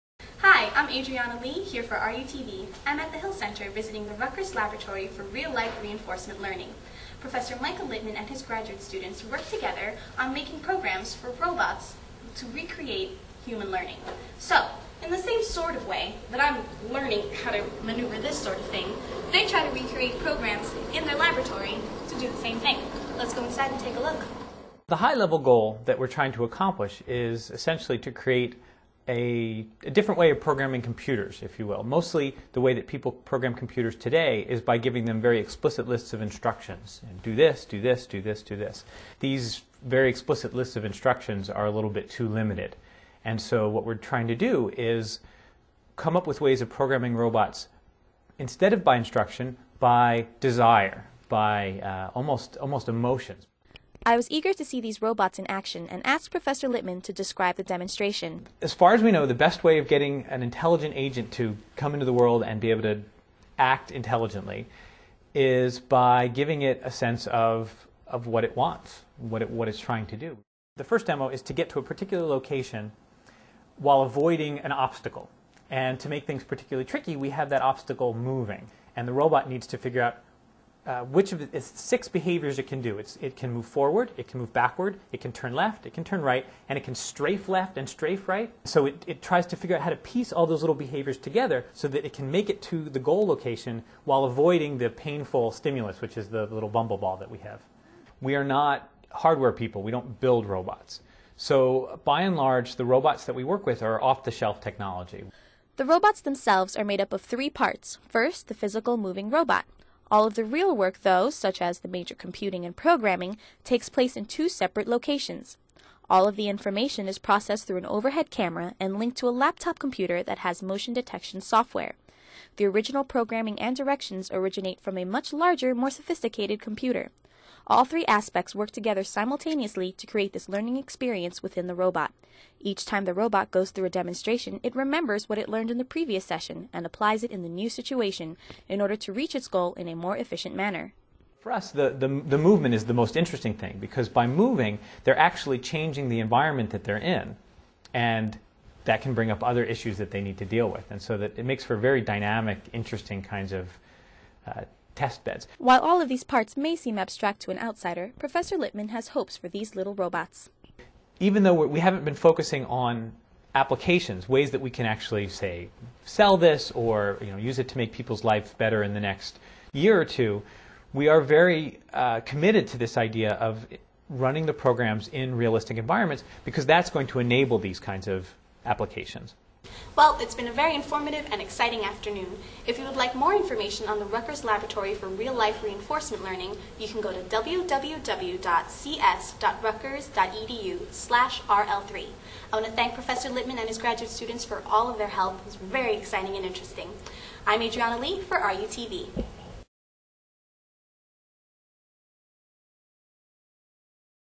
Robot Lab Interview
A piece from Rutgers TV (RUTV) about the lab that included a live learning demo and several robots (Aibo, Mindstorms) and an interview with Michael Littman.